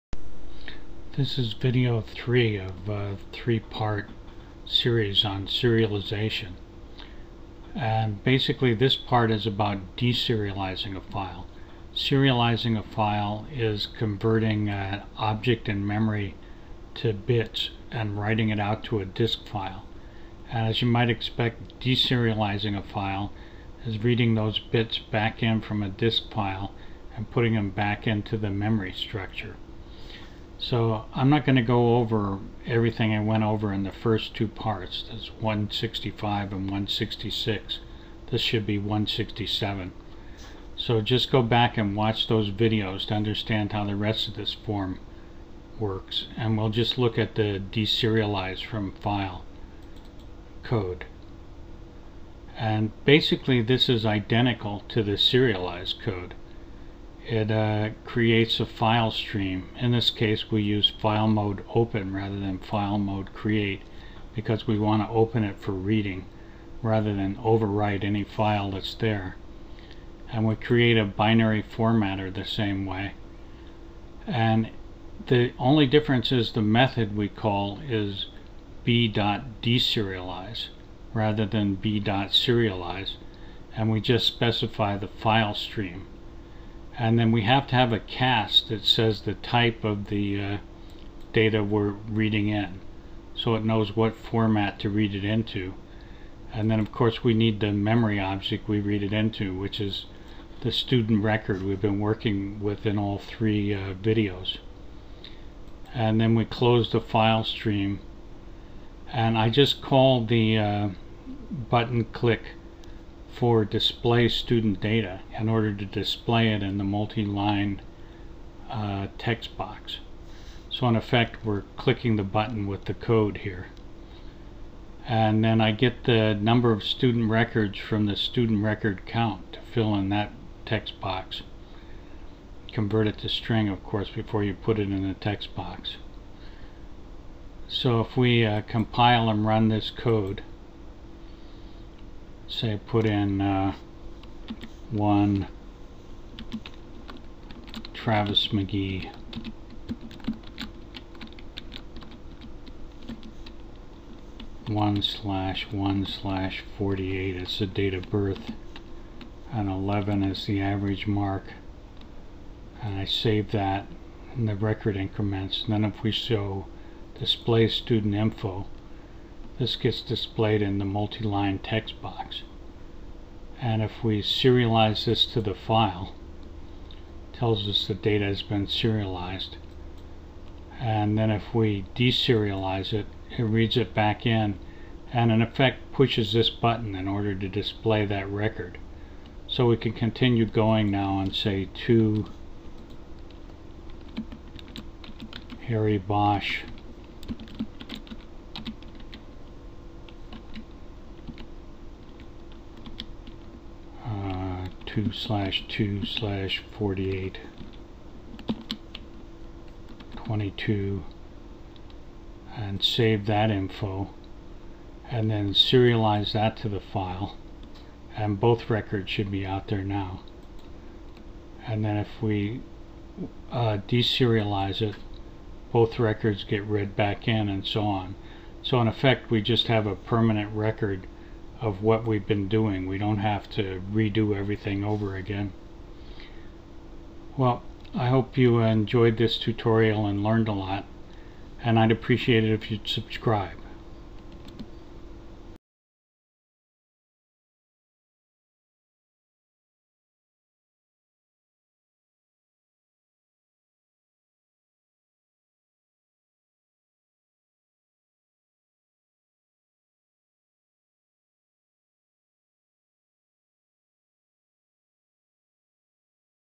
C# Edge 167 Serialization 3 DeSerialization
has all my video tutorials usually with source code in an organized cyber-library. In this video we learn how to deserialize data from a serialized binary data file back into a memory data structure, specifically a Generic List of Collections defined by a record structure class.